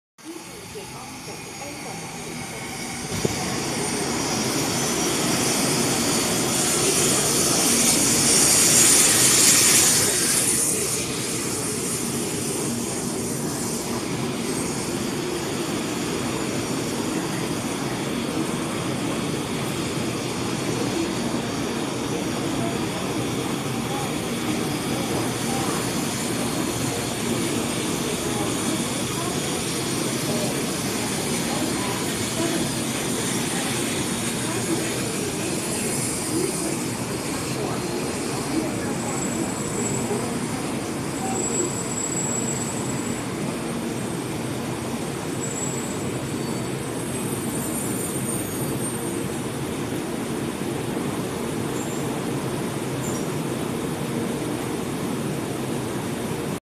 台灣高鐡扺達板橋站
台灣高鐡 THSR 700T ，設計源自 日本新幹線700系列車